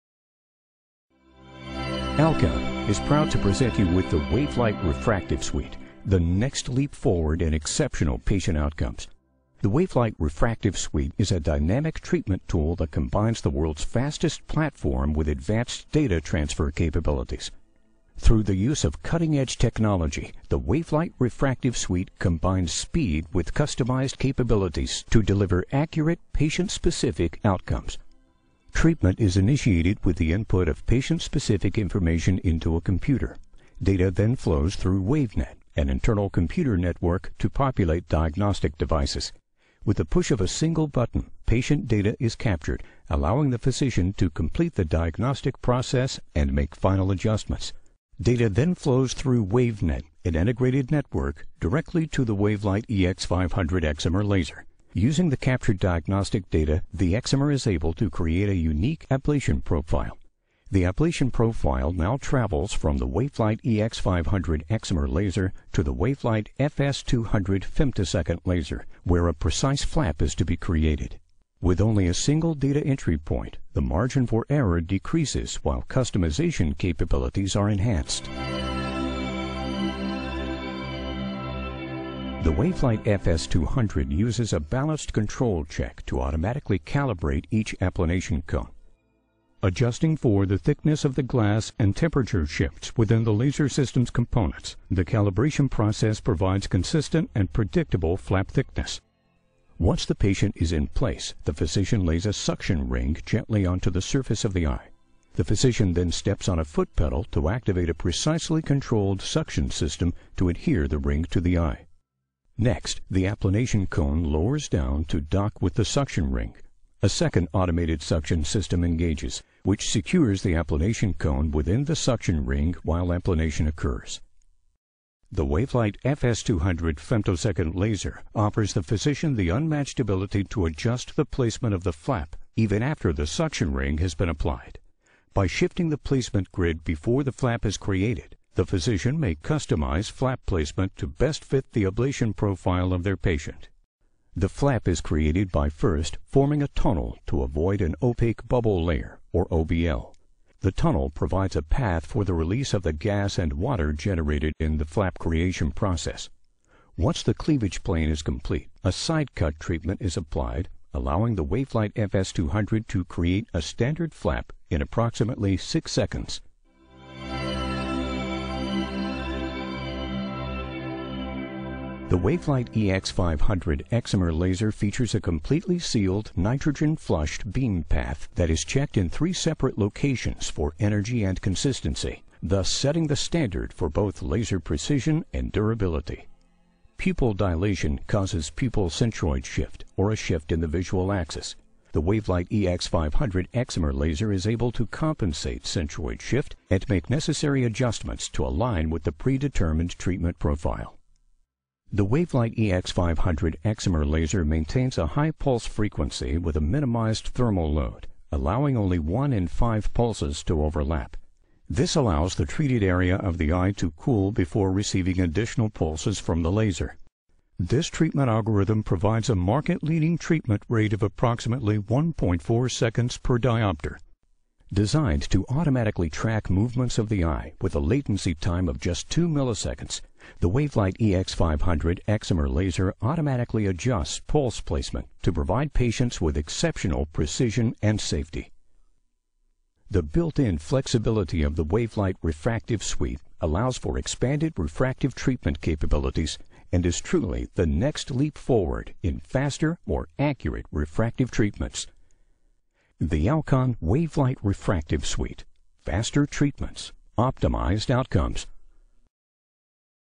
Alcon Wavelight refractive suite.wmv
Présentation de la nouvelle suite réfractive Alcon Wavelight, associant le laser femtoseconde FS200 et le laser excimer EX500.